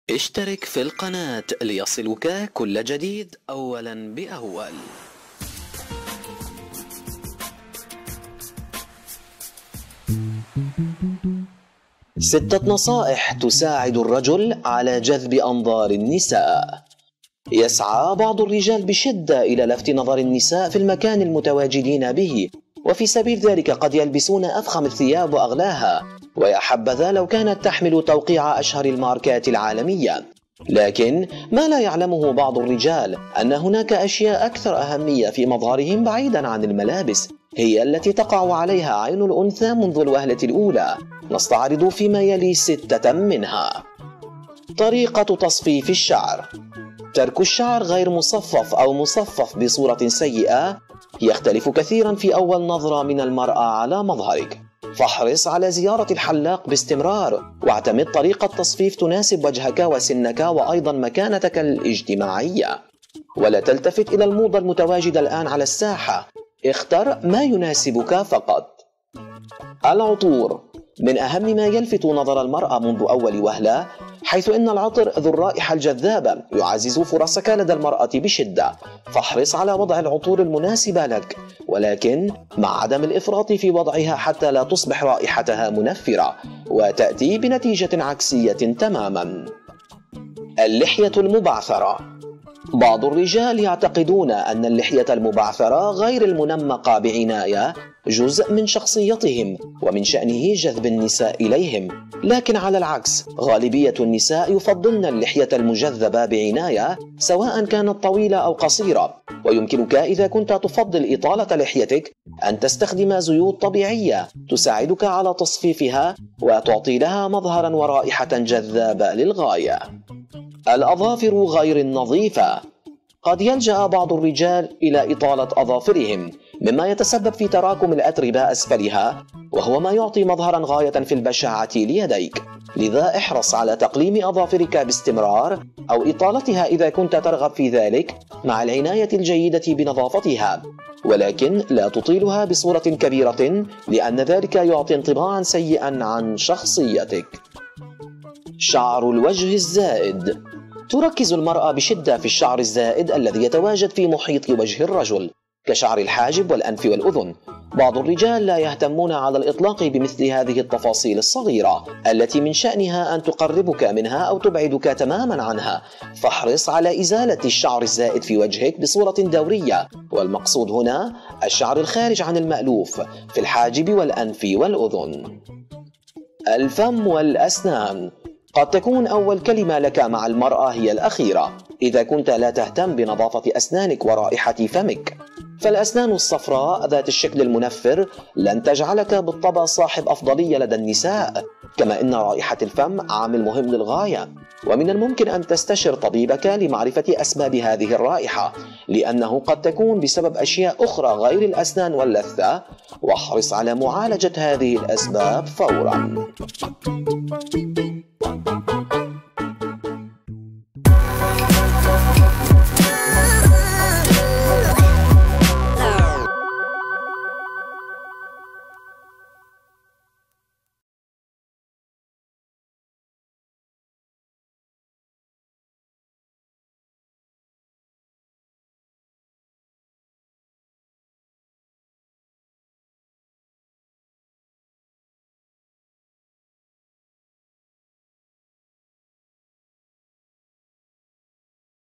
كيف تجذب النساء إليك ستة نصائح ستغير حياتك العاطفية
كيف تكون جذاباً للنساء ودونجوان عصرك وزمانك ؟
هناك الكثير من النصائح في هذا الشأن التي سوف تساعدك في تحسين مستوى جاذبيتك للبنات ولفت الأنظار إليك وفي هذا الفيديو سنقدم لك بعضاً منها
مشاهدة ممتعة :)